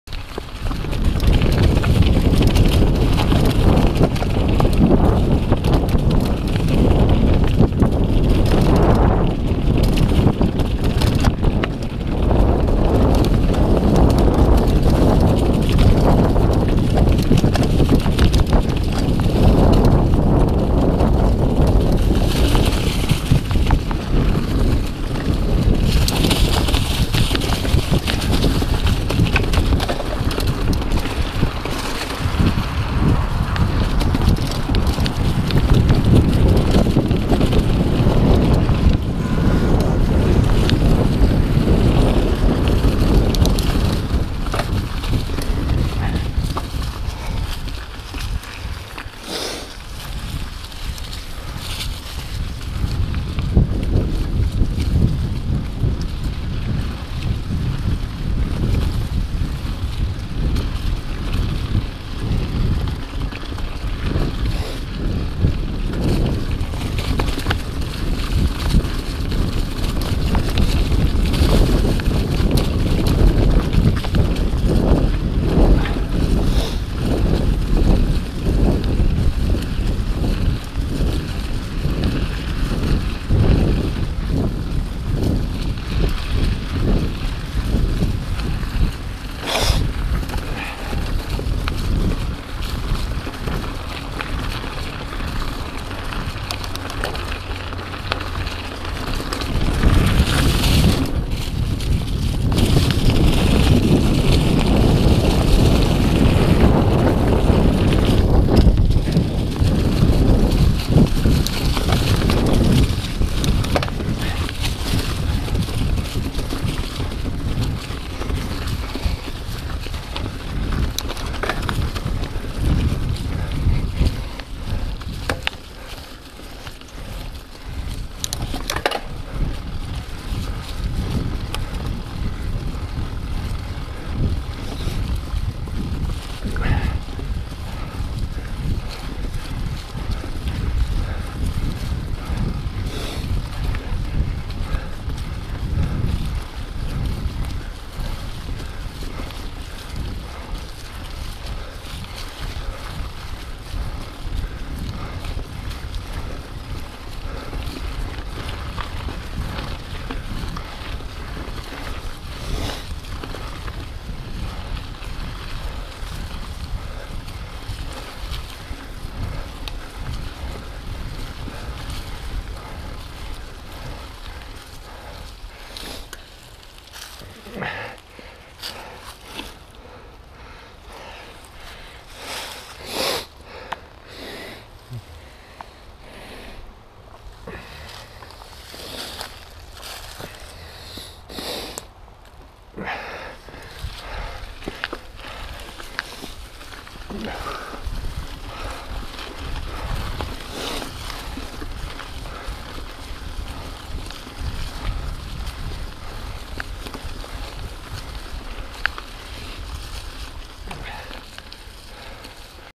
Trail 5, Governor Dick Park, Mt Gretna PA
trails 5 decent till my battery died. super long and super fast!